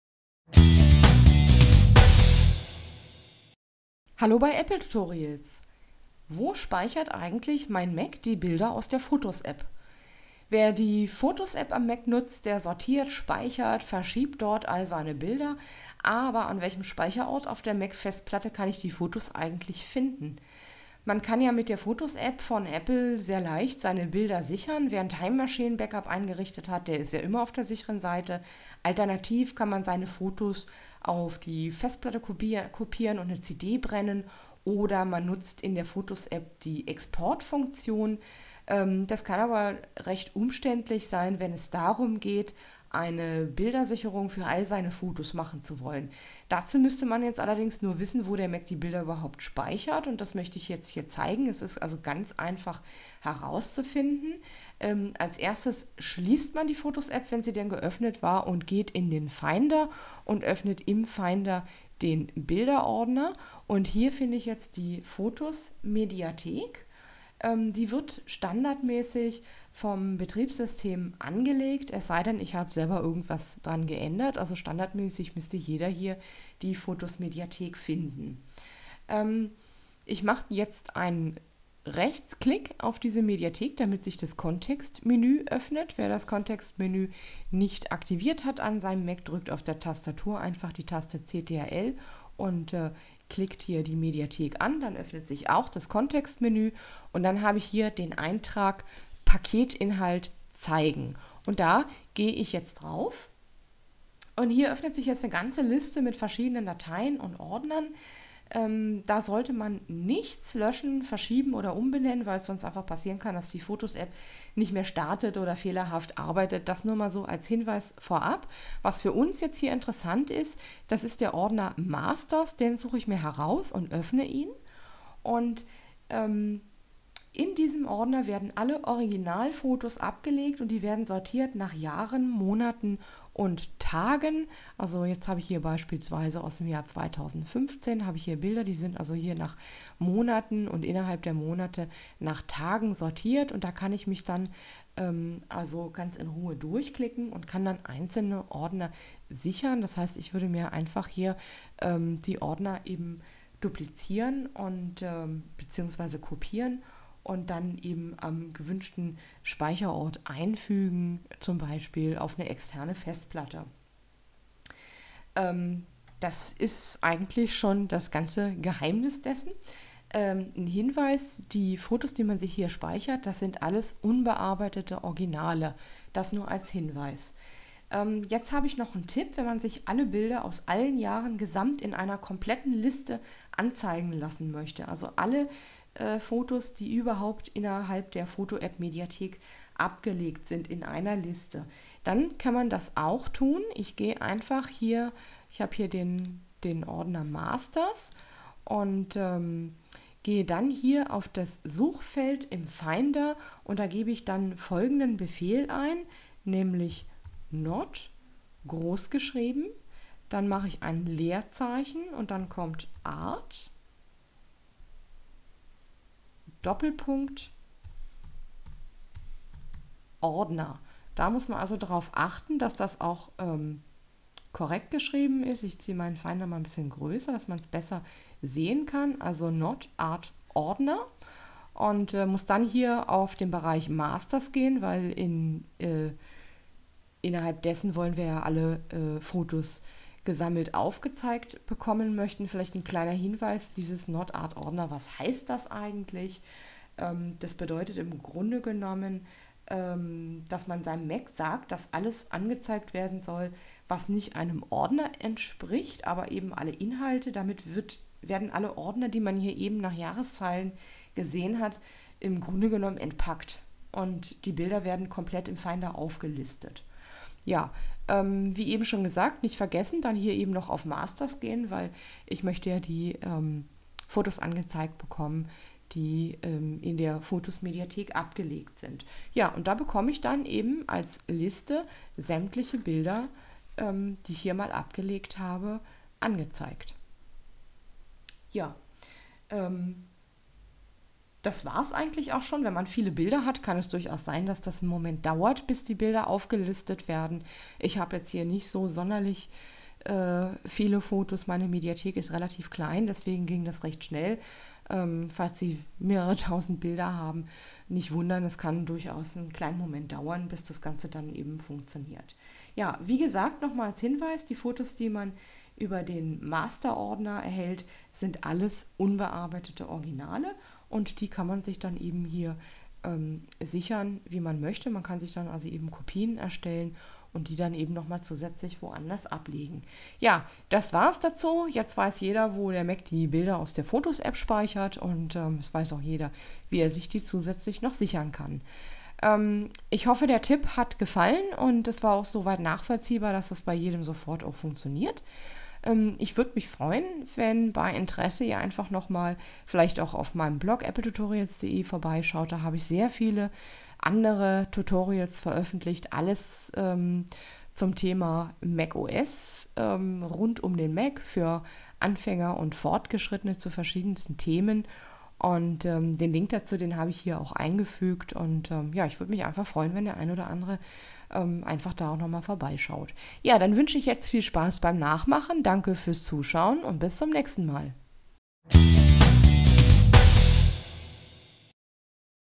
Wo speichert mein Mac eigentlich die Bilder aus der Fotos Mediathek?
Wenn man eine Sicherung der Fotos Mediathek am Mac vornehmen möchte, sollte man natürlich auch wissen, wo man seine Bilder findet. Im Video Tutorial zeige ich, wo man die Mediathek findet bzw. in welchem Ordern sich die Originalfotos befinden und wie man sich Kopien erstellt, die man dann seperat als Kopie z.B. auf einer externen Festplatte ablegen kann.